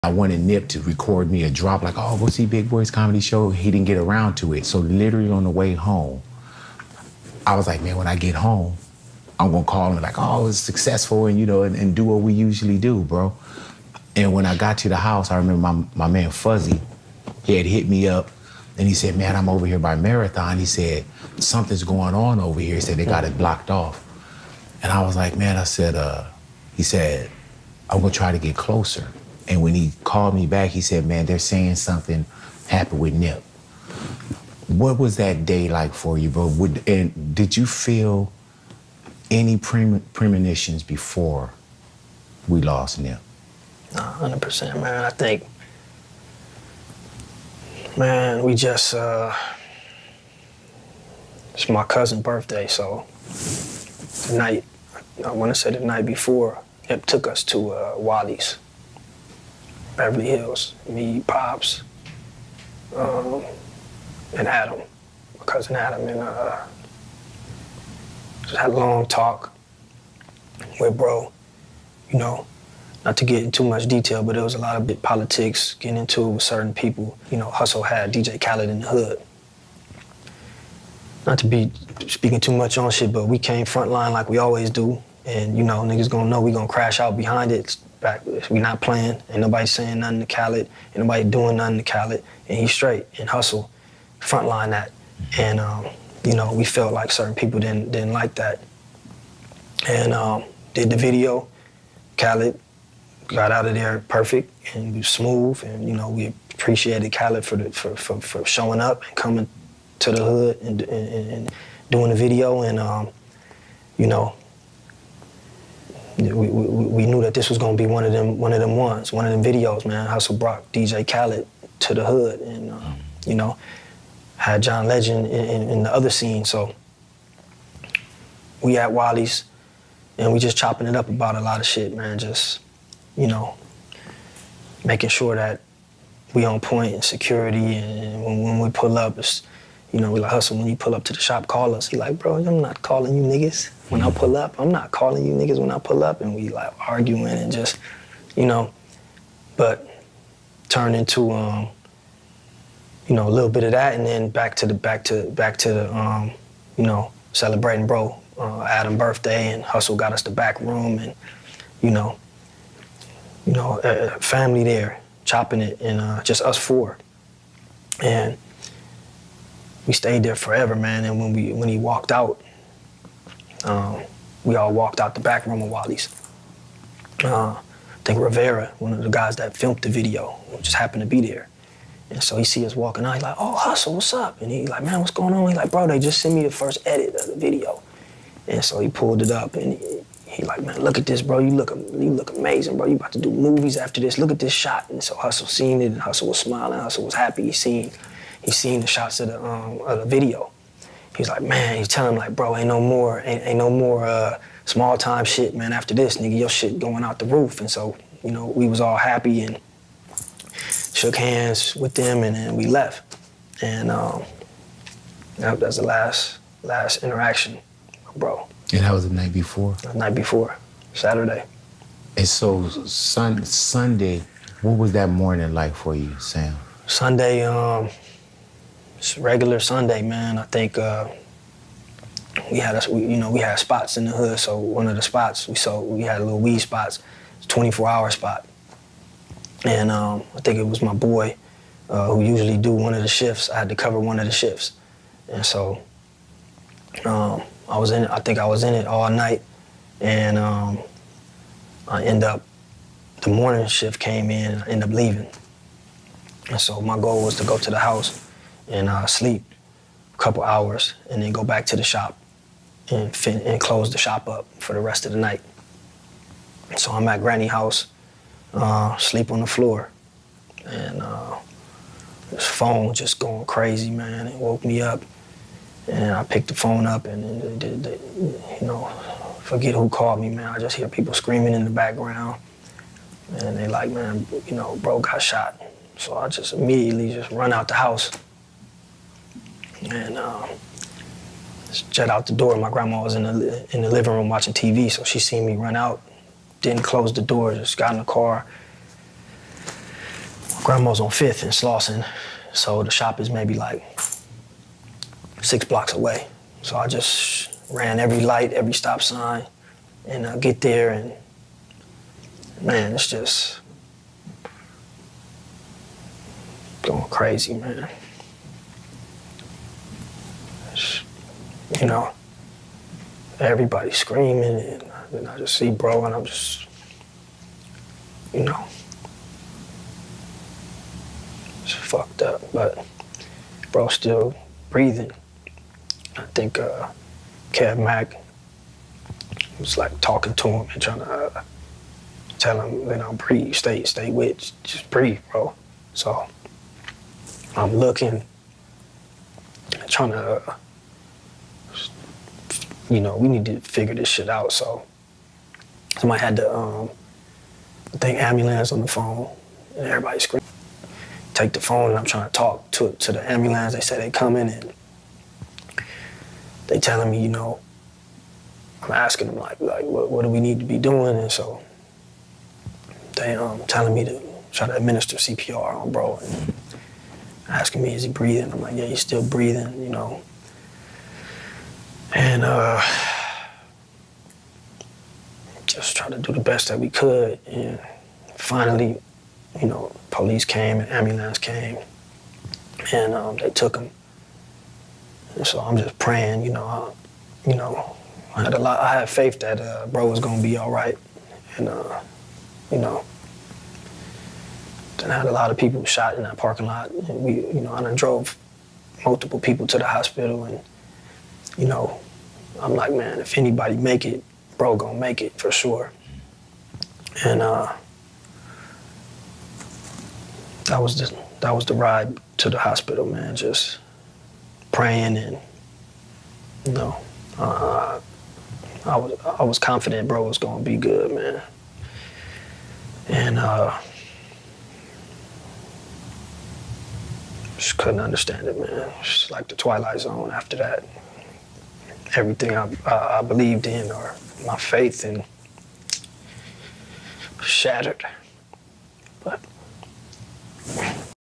Blacc Sam Reveals His Last 24 Hours with Nipsey Hussle
Blacc Sam, the Brother of Nipsey Hussle Opens Up about his last 24 hours with Nipsey Hussle
0:00 🕛 Big Boyd
2:30 🕝 Family Affair
4:30 🕟 Getting The Call